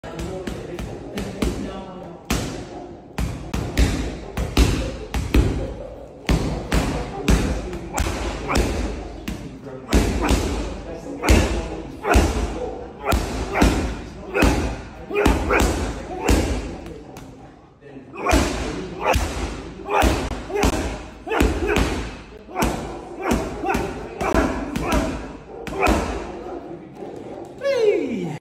PUNCH THE HEAVY BAG LIKE TEOFIMO LOPEZ 🥊
Teofimo Lopez showcases his boxing technique and power as he teaches us how to hit the heavy bag. He's held multiple world championships in two weight classes, including the World Boxing Organization (WBO) and Ring magazine light welterweight titles since June 2023; previously he held the unified World Boxing Association (WBA) (Super version), International Boxing Federation (IBF), WBO, and Ring lightweight titles between 2019 and 2021. Lopez has also held the lineal championship at lightweight and light welterweight. As an amateur, he represented Honduras at the 2016 Summer Olympics.

Lopez (19-1, 13 KOs) is coming off a banner unanimous decision win over Scotland’s Josh Taylor last month at The Theater at Madison Square Garden in New York City. With the victory, Lopez was awarded Taylor’s WBO title. What’s more, it rejuvenated the career of a troubled, young fighter who at one point seemed to be on the verge of fizzling out.

But immediately after the win over Taylor, Lopez insisted he would retire from the sport. Lopez subsequently declared he would relinquish his title. However, last week, the Brooklyn native informed the WBO that he would hold onto his belt, effectively ending all speculation about his future.

Now Lopez, 25, could be on track for another meaningful fight in a division that is brimming with meaningful opponents.